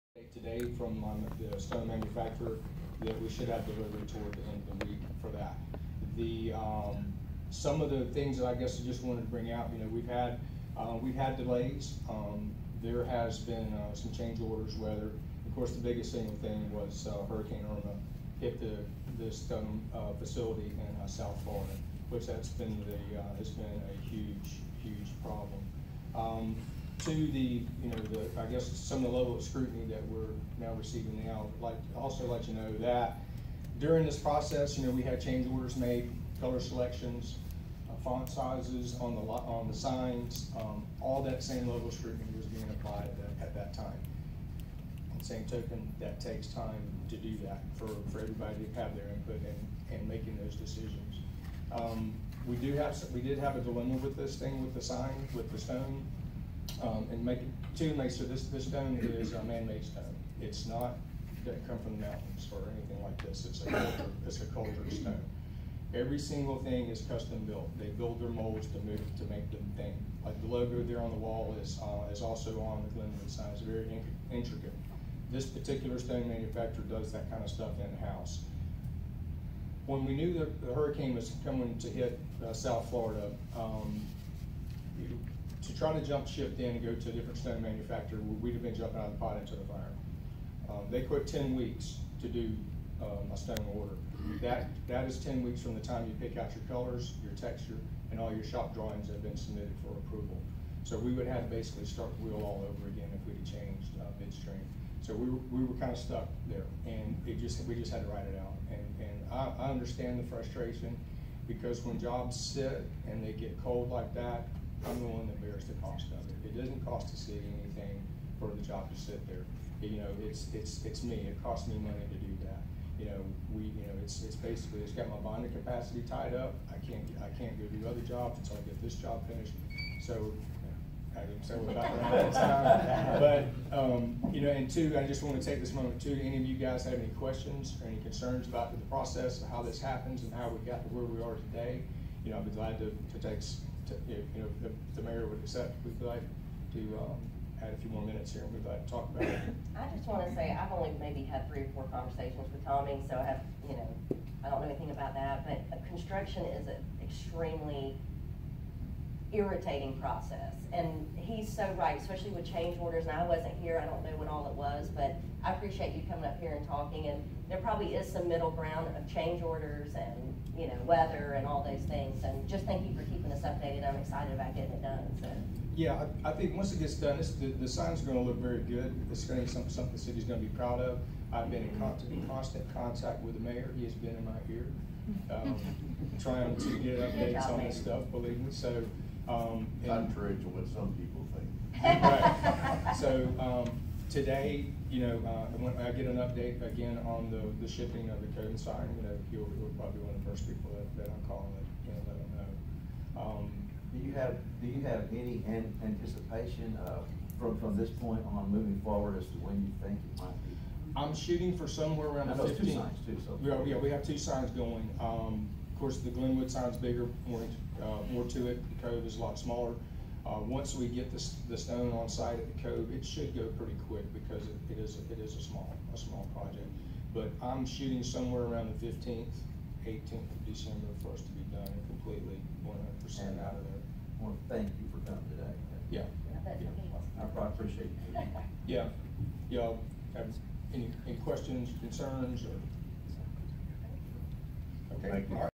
Update on Cove and Glenwood signs
Tommy Hamm, of Winterfell Construction, updates the City Commission on the progress.